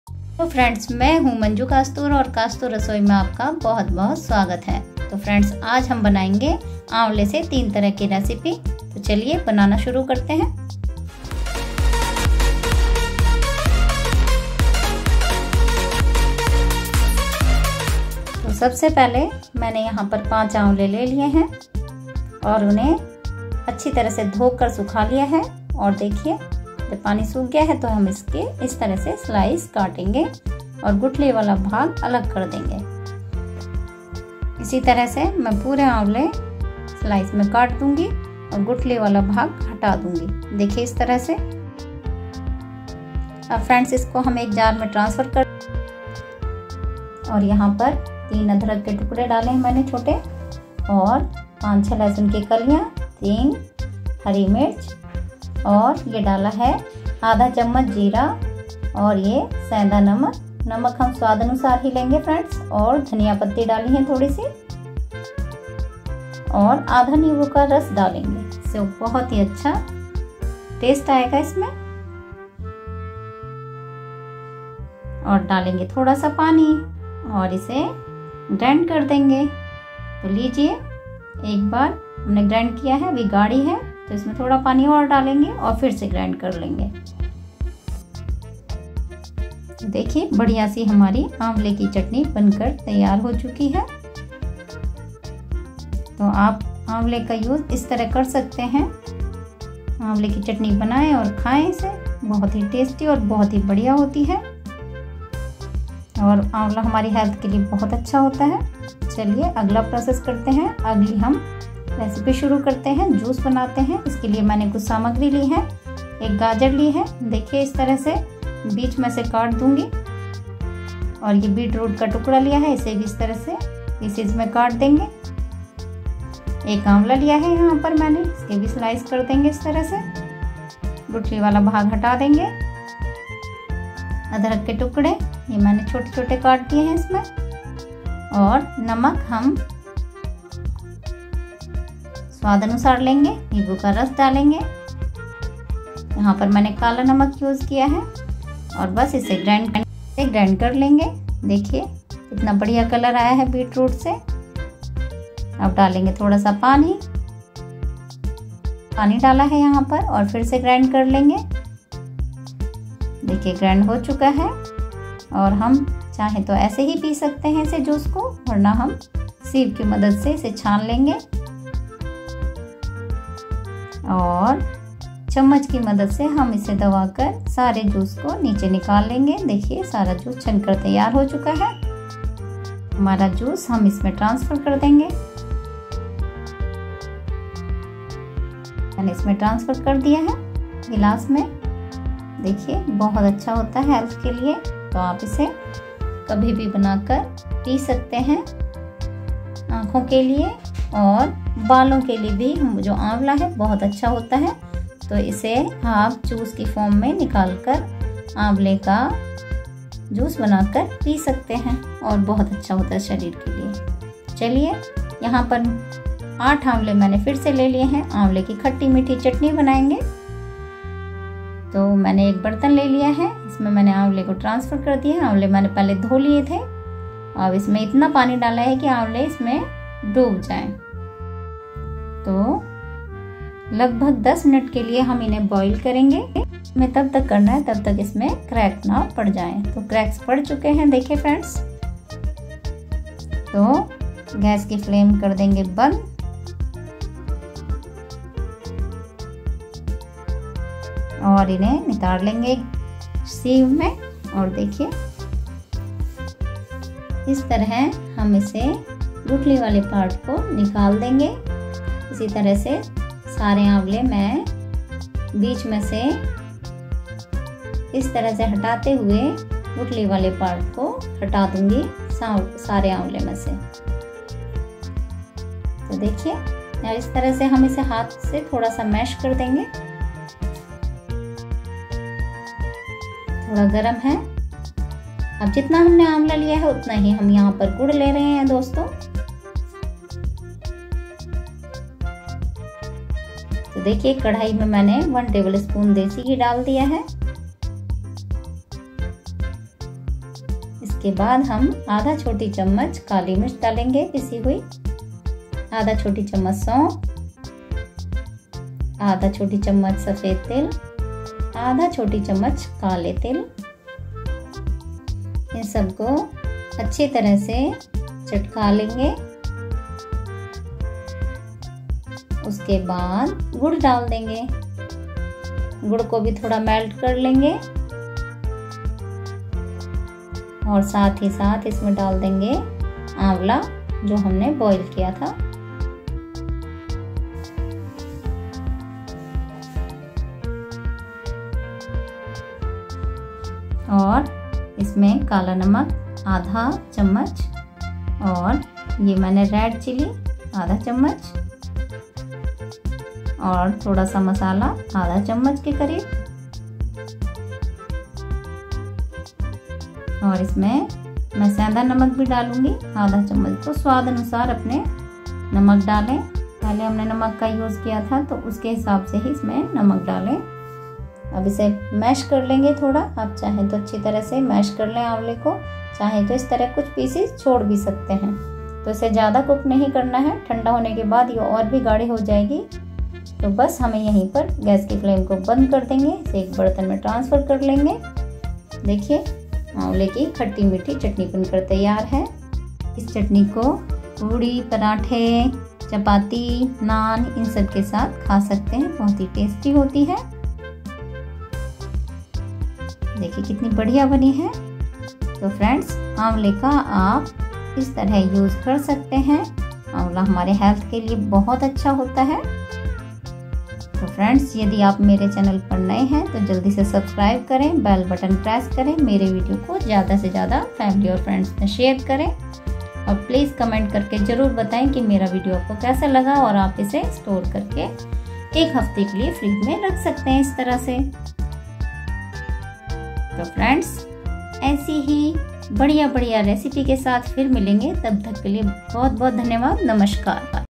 3 Amla items in one video by Kastoor Rasoi
3 Amla items in one video by Kastoor Rasoi

About this video: Hello friends, see 3 items of Amla in this video. You can make them very easily. Health and tasty Chutni and Healthy Juice.